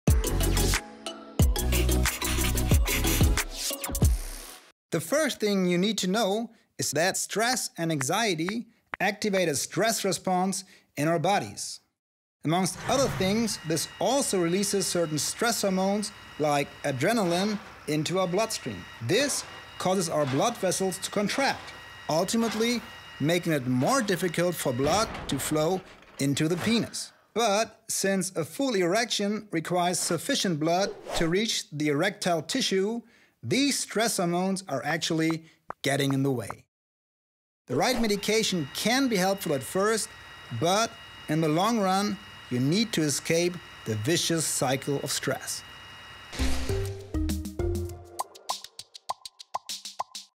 Sexual Problems | Chronic Stress Leads to Erectile Dysfunction | Oneindia News
Everyone experiences stress. Our bodies are designed to feel stress and react to it. It keeps us alert and ready to avoid danger. But, when stress persists, the body begins to break down and problems like erectile dysfunction can occur. The key to coping with stress is identifying those conditions in your life causing the stress and learning ways to reduce them.